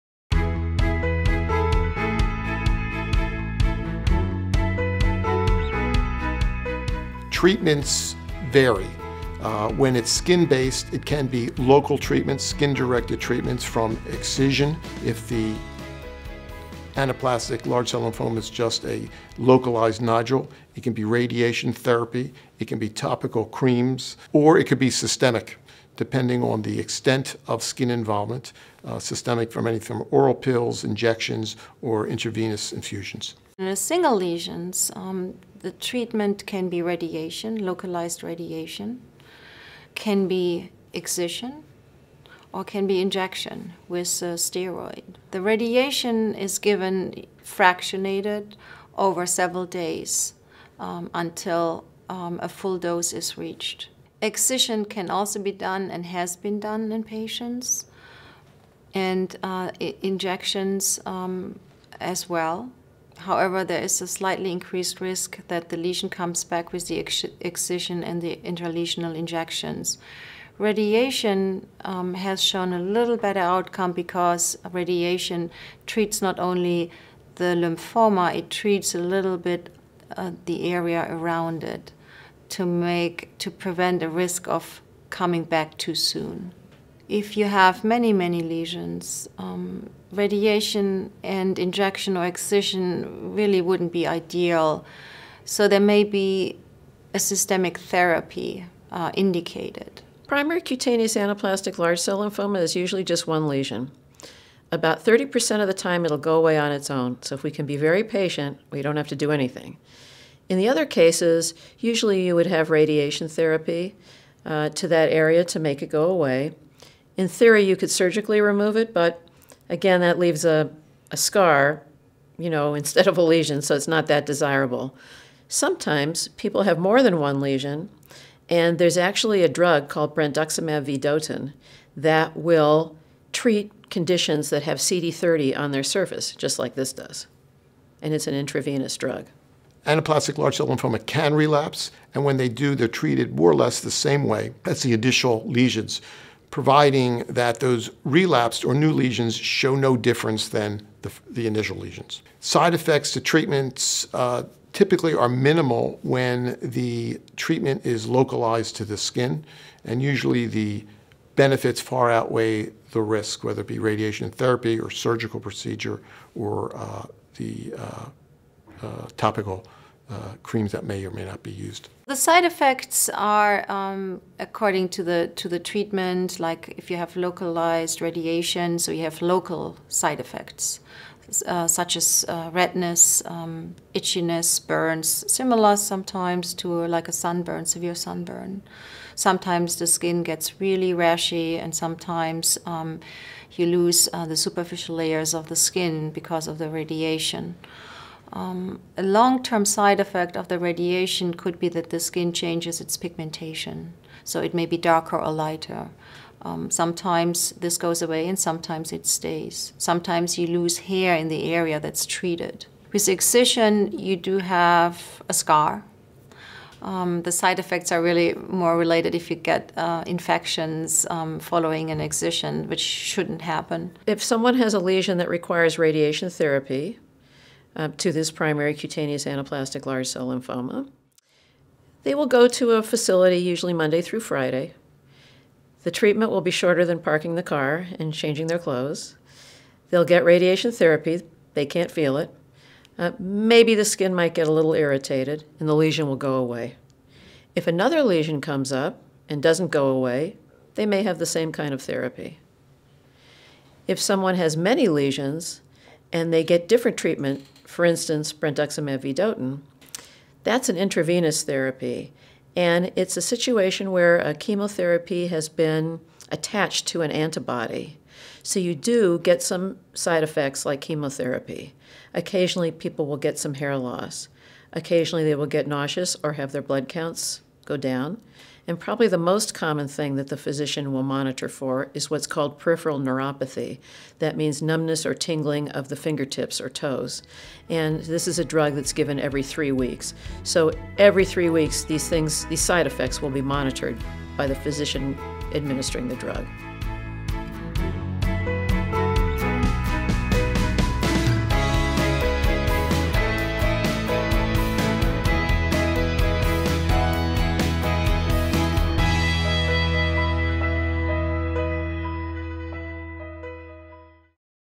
Answers from the Experts: ALCL Treatment
Cutaneous lymphoma experts provide answers to the question: "Primary Cutaneous Anaplastic Large Cell Lymphoma: What are the treatment options and side effects?"